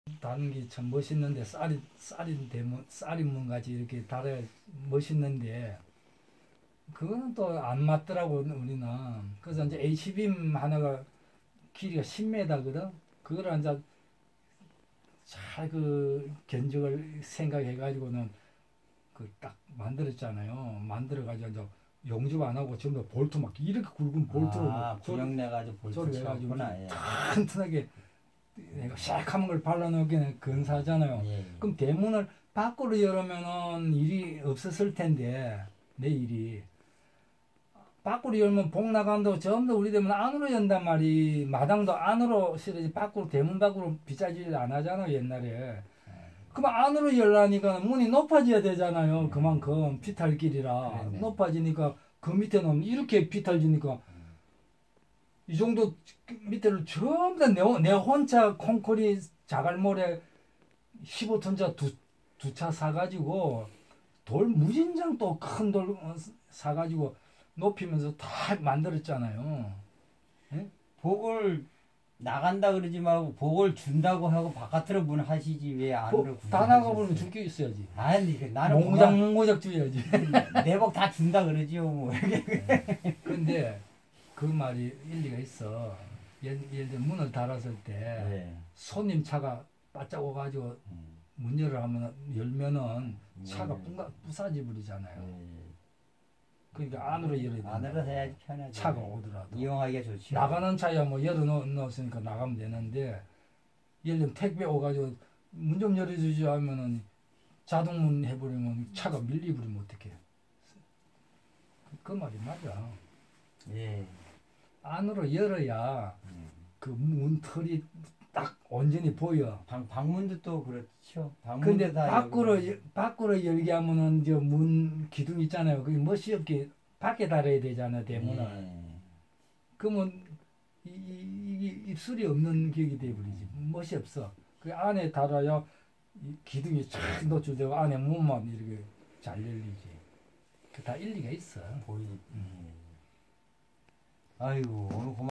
무운(無耘)선생 이야기 4187/ 대문을 안으로 열게되는 이유와 장점
옛날 선조들은 "대문을 안으로 열어야 복이 나가지 않는다"라고 하였다.
대문을 안으로 열게 설치하면 우선 대문 기둥이 밖에서 보면 전체가 노출되어 보기좋다. 그리고 택배등 차량이 가까이 왓을 때 자동문이라면 차량이 파손되기도 한다 그래서 대문은 안으로 열게 만들었다.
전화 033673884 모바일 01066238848